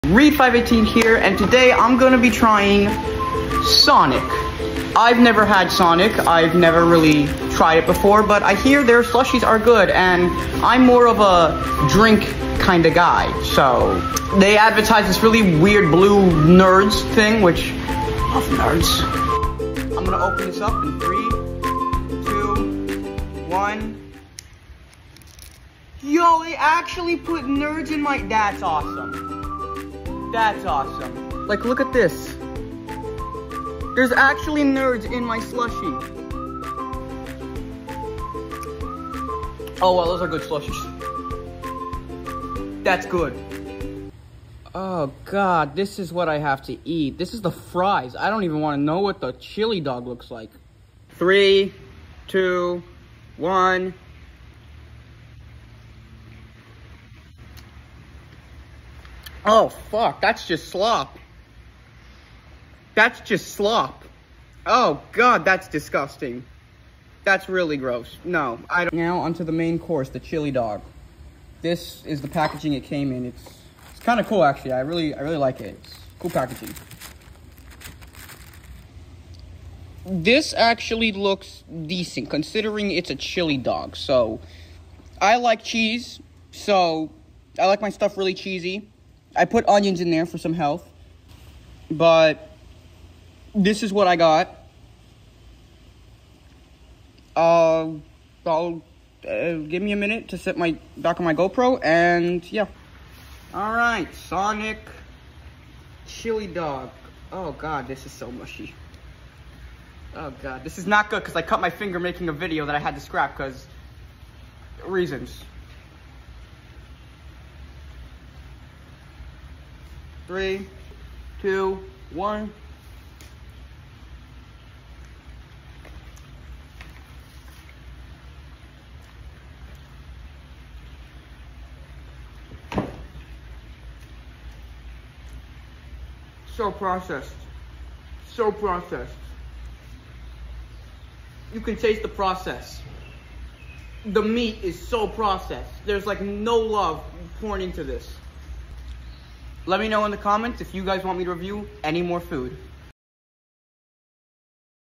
Is SONIC Actually bad!? (Might surprise you)
In this video I decided to try sonic. I’ve never had it before. So I really wanted to try it.